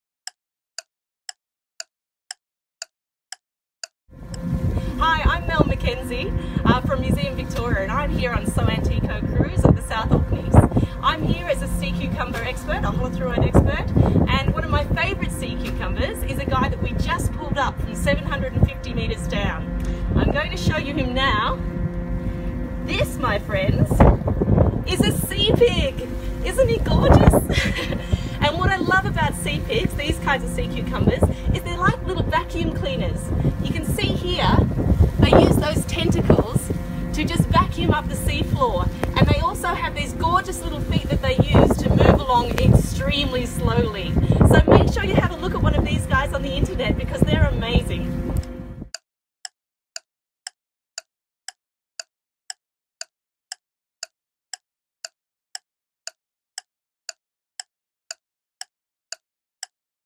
Polar Science in 60 Seconds | Antarctic Sea Pigs
We’ve challenged our science, engineering and support staff and their research collaborators to explain in 60 seconds what they do. Melanie Mackenzie shows off her latest catch - a sea pig!